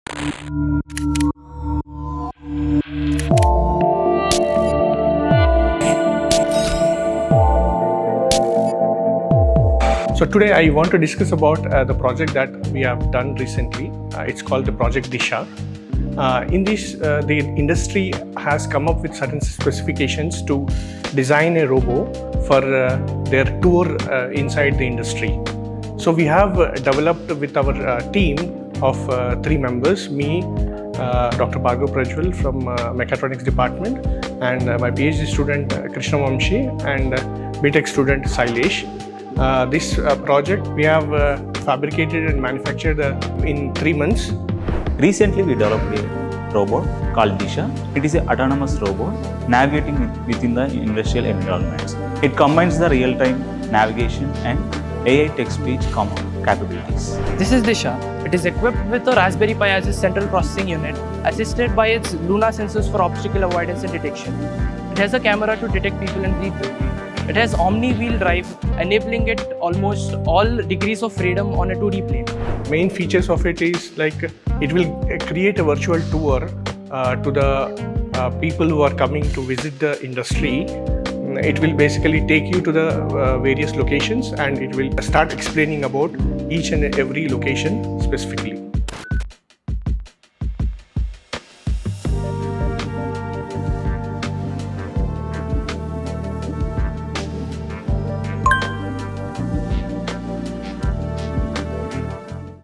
Meet Disha, an Autonomous Robot Developed at the AI & Robotics Studio, Woxsen University
Meet Disha, our autonomous robot designed to explore and navigate industrial environments! Developed by Dr. Bhargav Prajwal Pathri, Assistant Professor at the School of Technology, along with Ph.D. Scholar Krishna Vamshi Ganduri and B.Tech Student Sri Sylesh, Disha is not just a guide, it also provides insights about various locations in the facility.